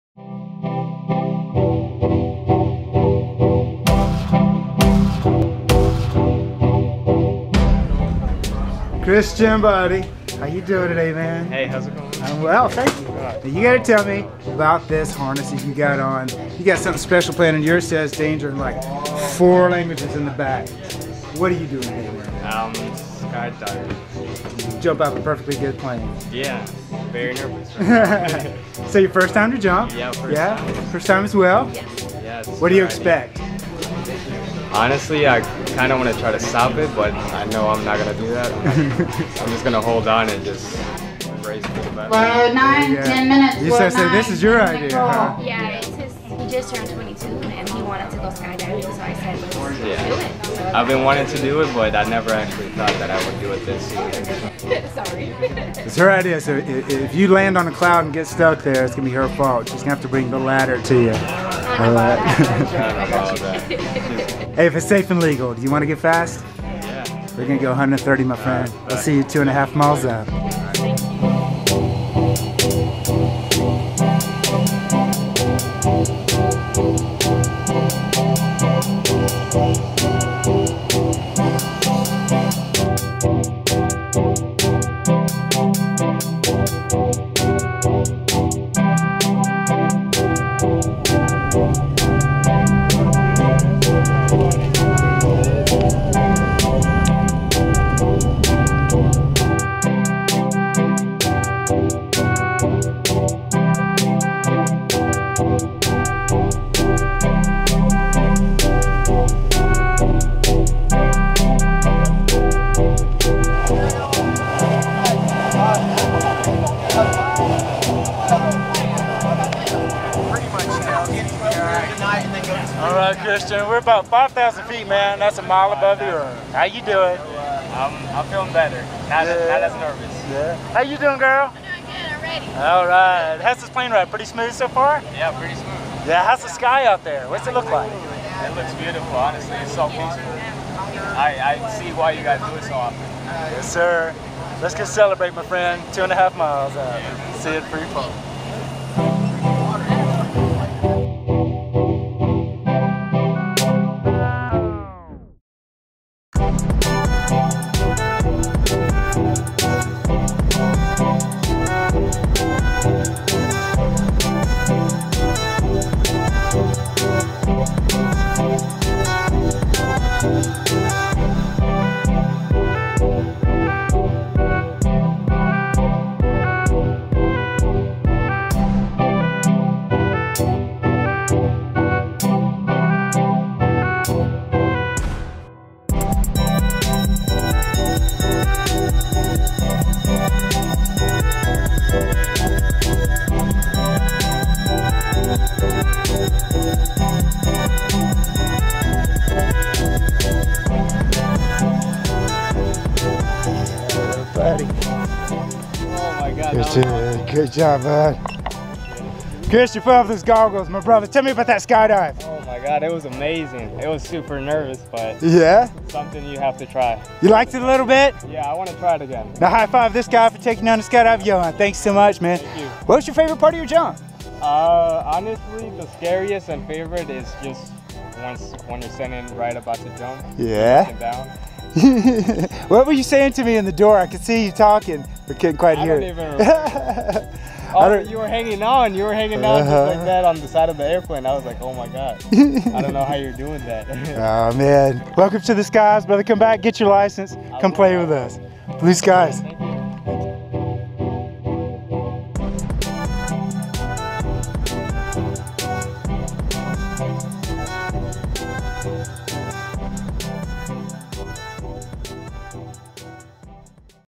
Tandem Skydiving! Christian from Norcross, GA at Skydive Georgia!
Another fun fantastic tandem skydive at Skydive Georgia! Watch Christian from Norcross, GA experience Tandem Skydiving with the best in the business!